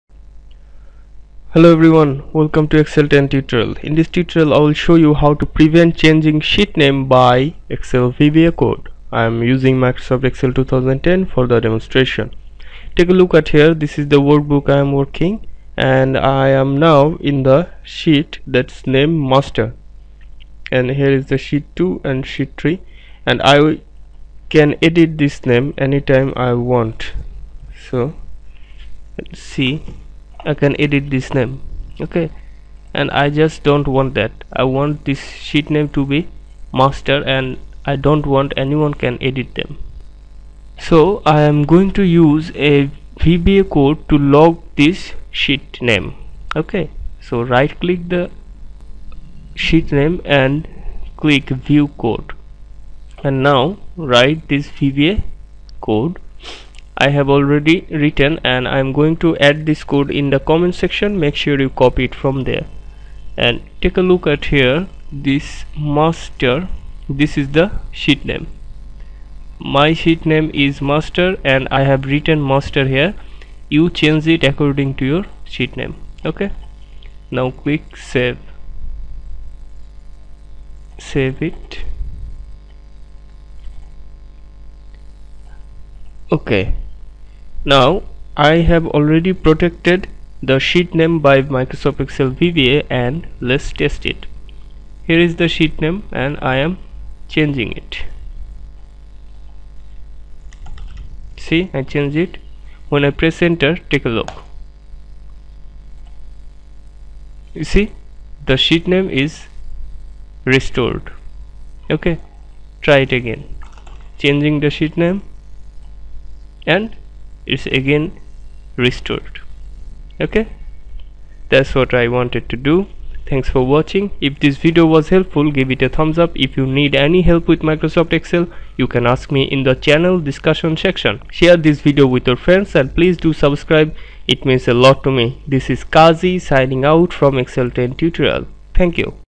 Prevent From Changing Sheet Name By Using VBA Code
Welcome to excel 10 tutorial and I am going to protect worksheet name in excel 2010. Actually I am going to prevent renaming sheet with excel vba.

Yes I will use Excel Vba for this one and I am using Microsoft excel 2010 for the demonstration. Let’s get started.

Step 1: Right click on the “Sheet name”.

Step 2: Click on “View code”.

it’s done.

You just protect worksheet name in excel 2010 using excel Vba.